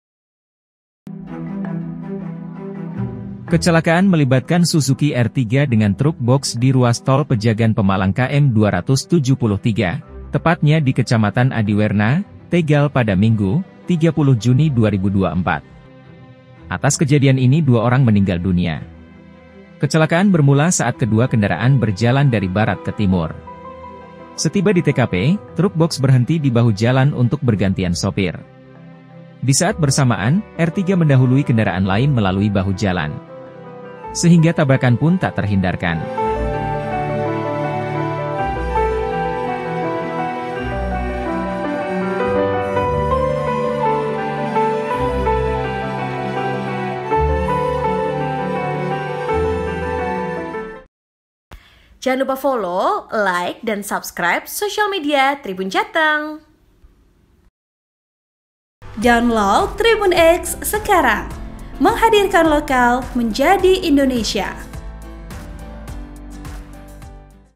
Kecelakaan Ertiga Seruduk Truk Boks Berhenti di Tol Tegal 2 Meninggal
Kecelakaan melibatkan Suzuki Ertiga dengan Truk Boks di ruas tol Pejagan-Pemalang KM 273, tepatnya di Kecamatan Adiwerna, Tegal pada Minggu, 30 Juni 2024.

Atas kejadian ini 2 orang meninggal dunia.

Kecelakaan bermula saat kedua kendaraan berjalan dari barat ke timur.

Setiba di TKP, truk boks berhenti di bahu jalan untuk bergantian sopir.

Disaat bersamaan, Ertiga mendahului kendaraan lain melalui bahu jalan.

Sehingga tabrakanpun tak terhindarkan.